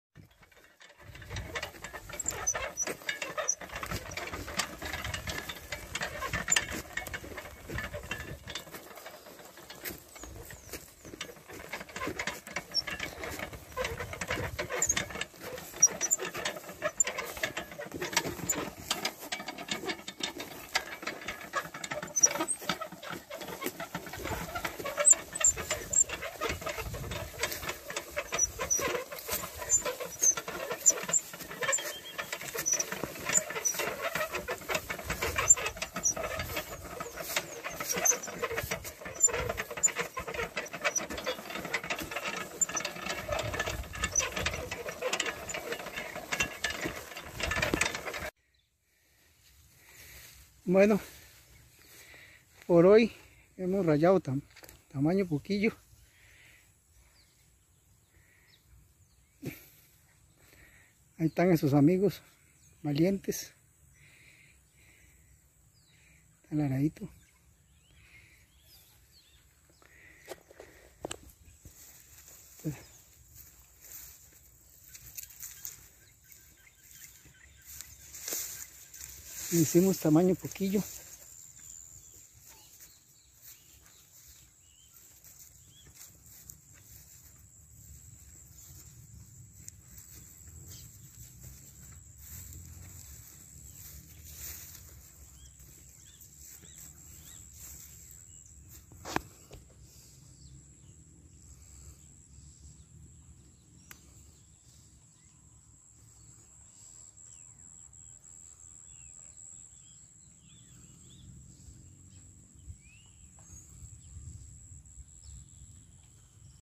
RAYANDO CON ARADO Y BUEYES... boyeros Aly Alvarado y Marvin Jarquín - Boyeotico CR 2023
Los boyeros agricultores don Aly Alvarado y don Marvin Jarquín rayan el terreno para la siembra de maíz... la yunta de Cóndor y Pizote jalan el arado...
el el norte, allá por Upala, pronto tendremos parto de elotes...
Ya casi huele a yoles, tortillas, tamal de maíz...